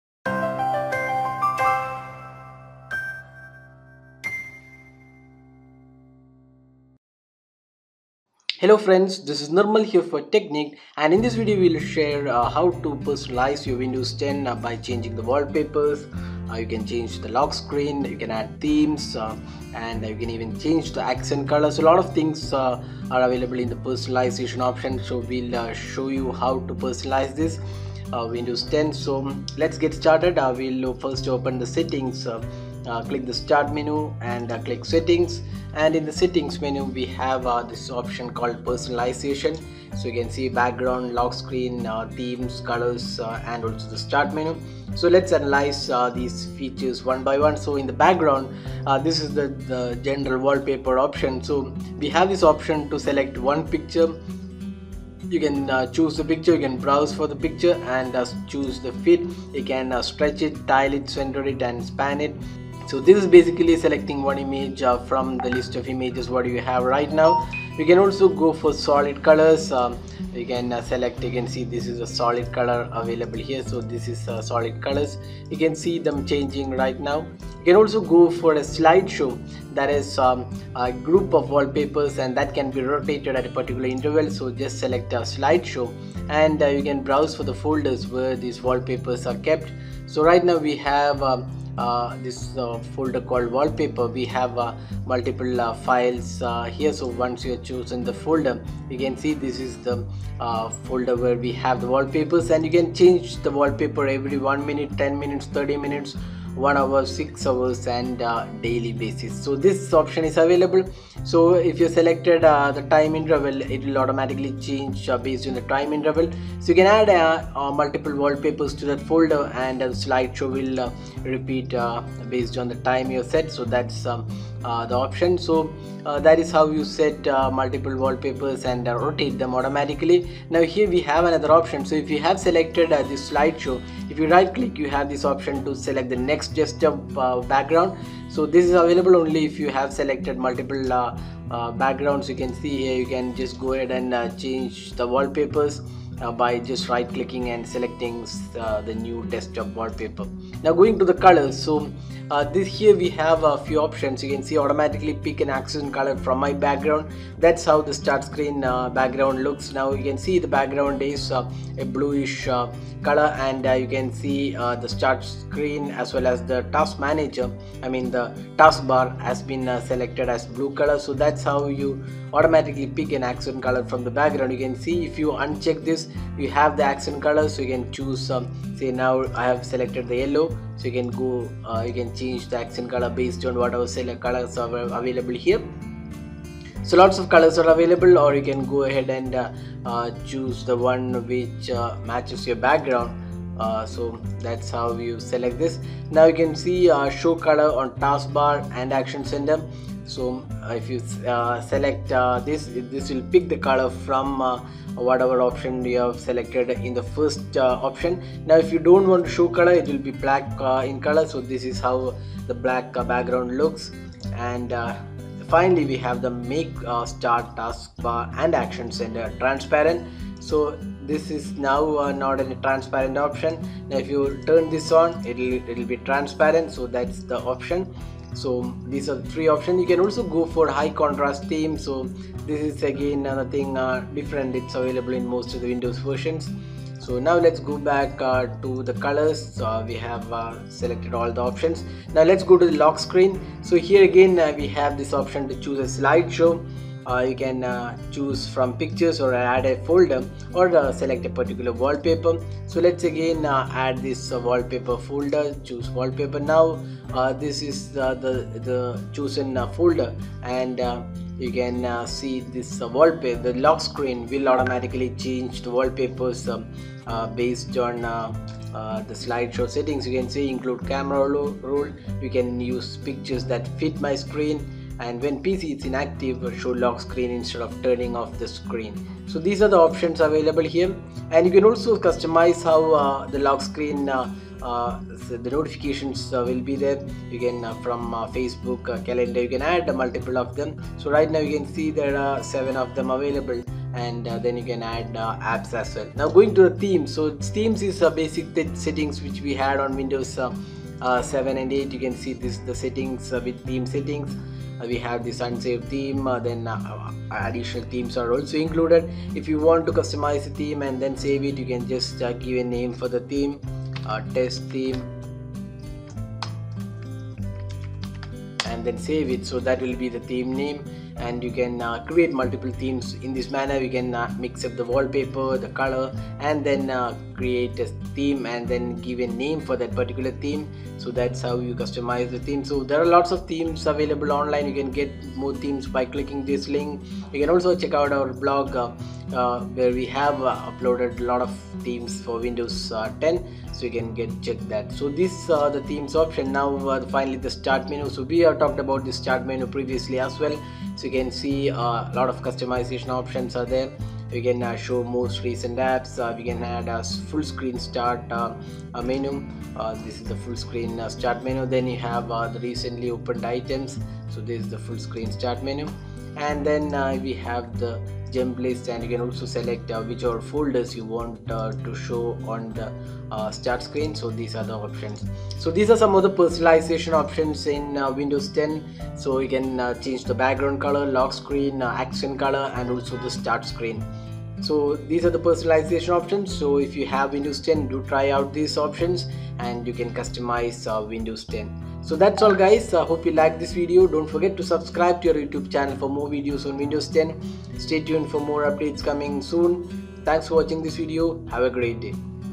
How to Change Wallpapers, Themes, Lockscreen and Start in Windows 10 | Techniqued
A quick video on how to personalize Windows 10 by changing Wallpapers, lock screen, themes and accent colors. Windows 10 does offer lots of options to customize.